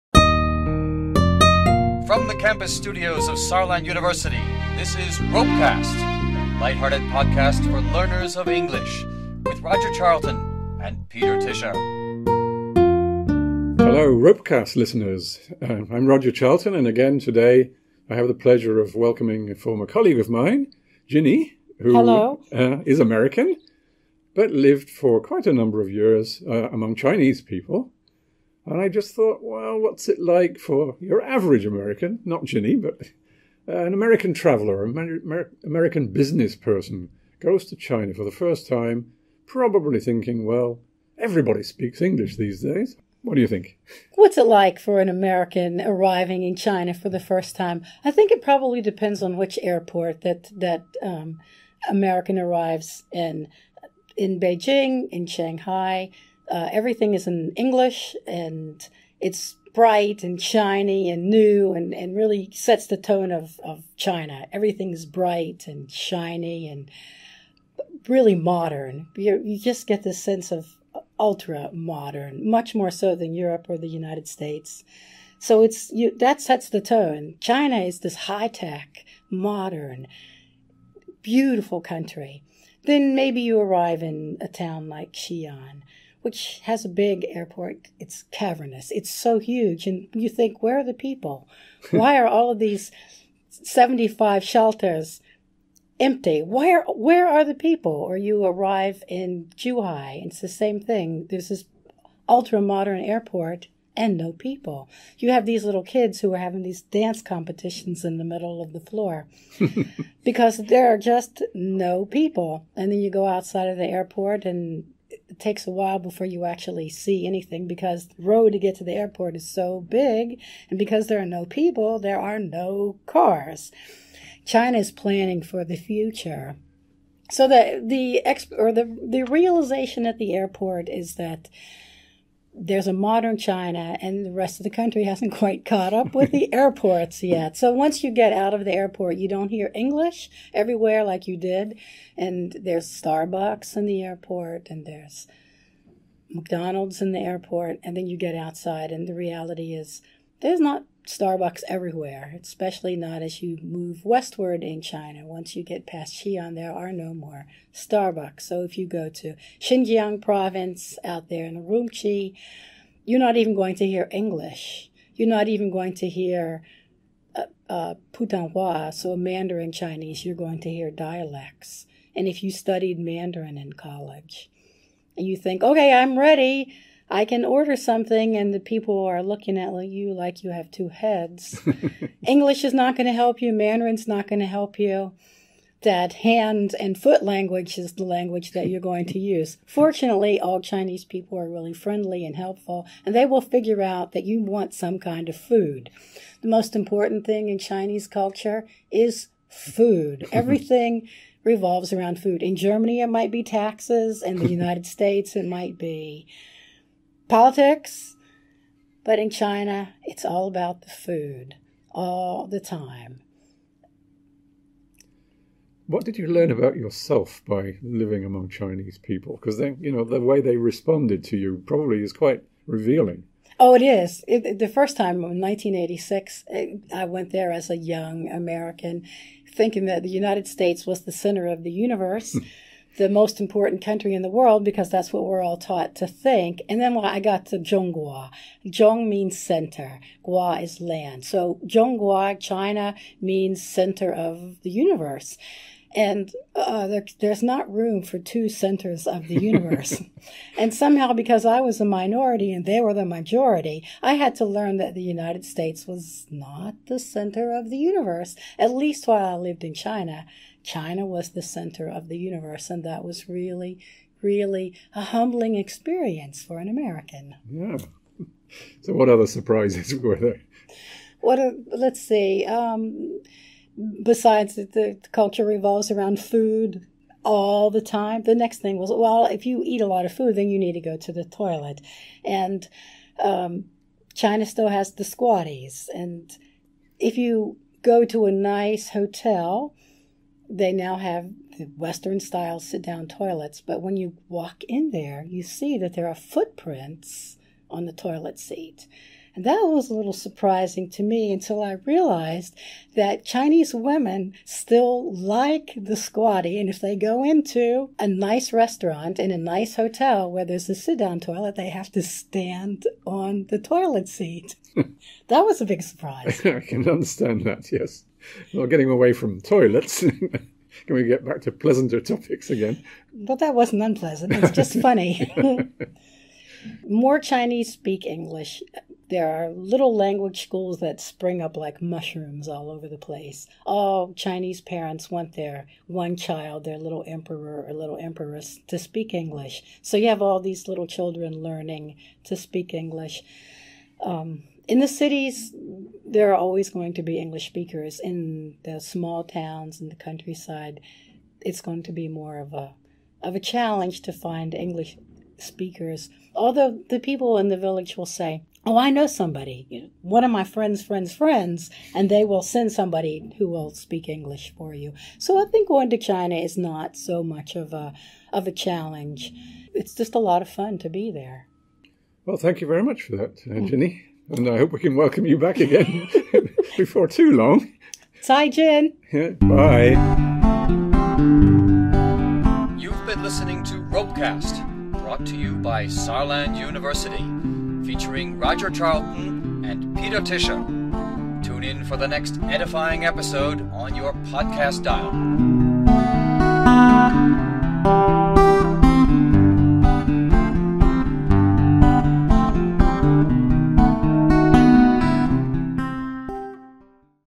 A journey to the centre of the universe (RoPeCast #127)
Why not join Roger and his colleague Ginny on a journey to the centre of the universe?

Ginny shares her impressions of what it's like when you first come to China as a westerner, when English eventually fails as lingua franca and so does standard Chinese, i.e. Mandarin.

But not to despair, people who live in the centre of the universe will know what you need. It's food and always food in China.

That much at least they do have in common with the Saarlanders producing this podcast! ;-)